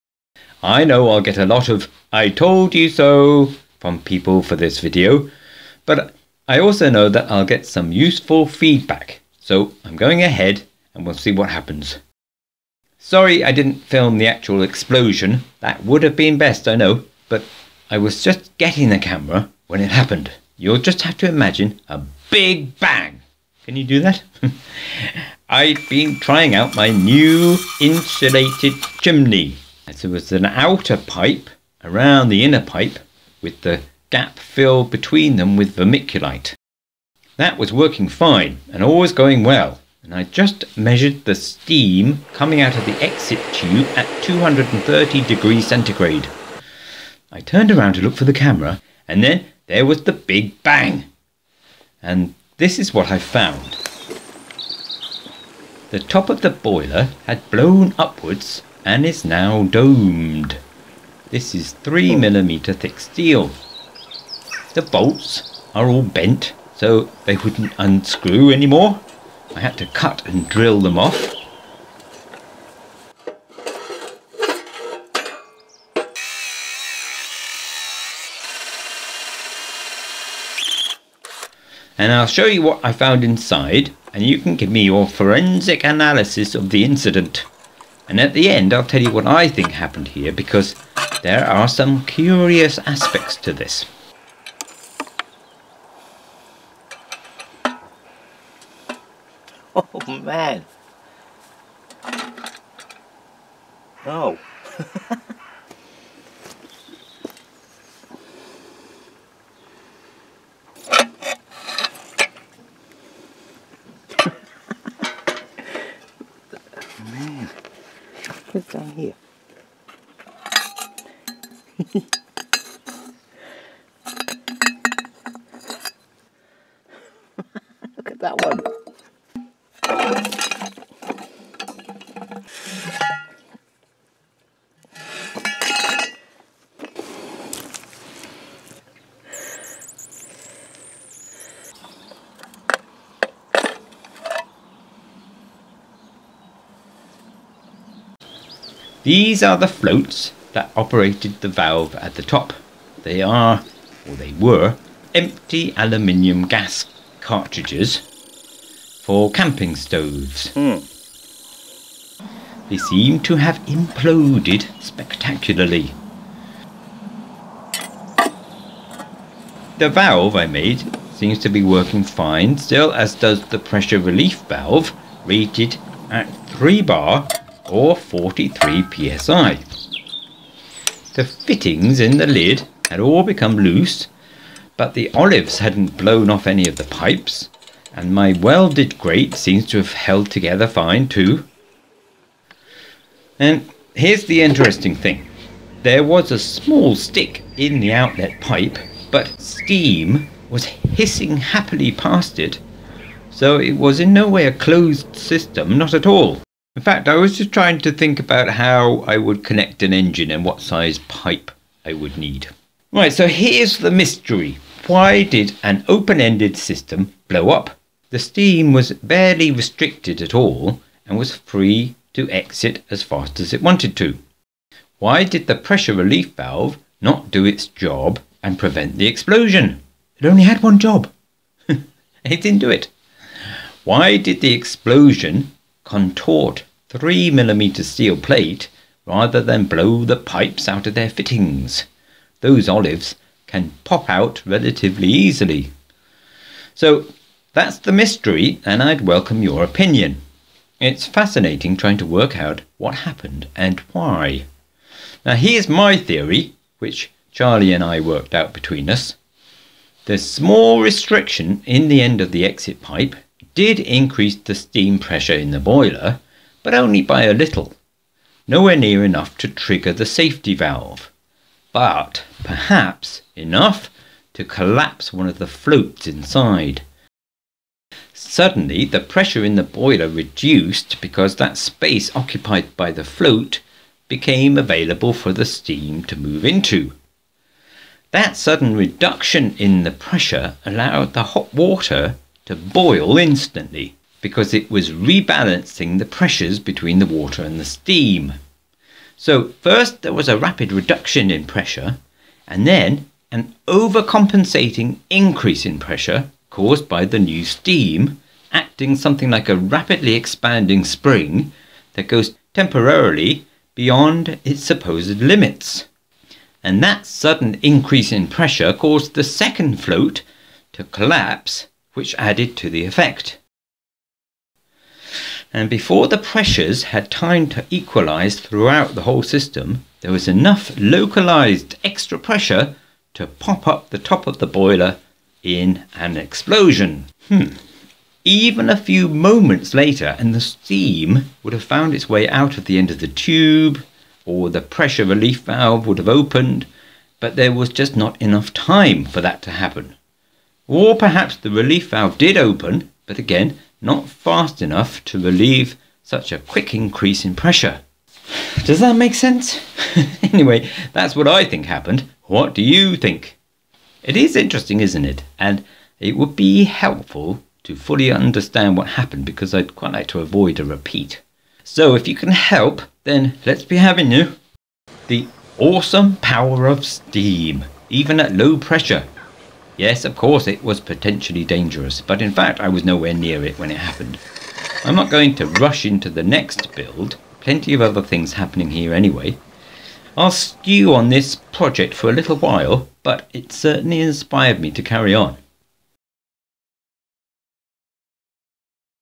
Why Did My Boiler Explode??!! My Own Big Bang Theory...
Here’s a mystery for you to solve if you like – why did my flash steam boiler explode?